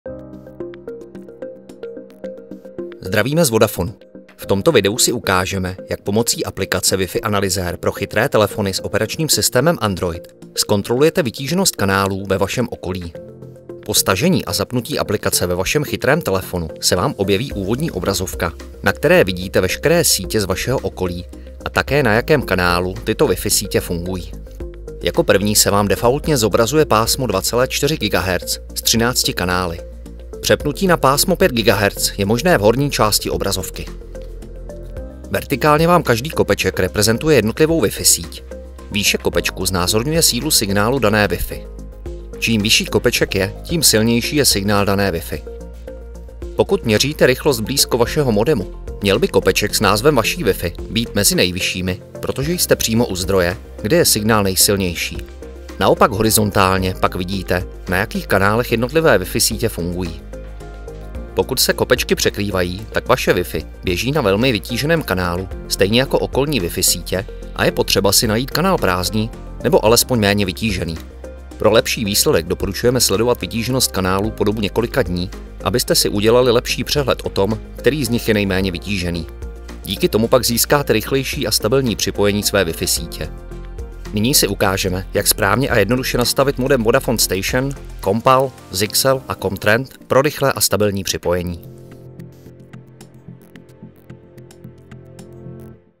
Měření vytíženosti Wi-Fi kanálů v okolí pomocí aplikace pro chytré telefony s OS Android
Ve videu vám ukážeme, jak zjistíte, jestli vaši Wi-Fi síť neruší někdo jiný v okolí a jak vybrat vhodný Wi-Fi kanál, aby vám připojení hladce fungovalo. Další informace k Wi-Fi kanálům najdete také v Centru péče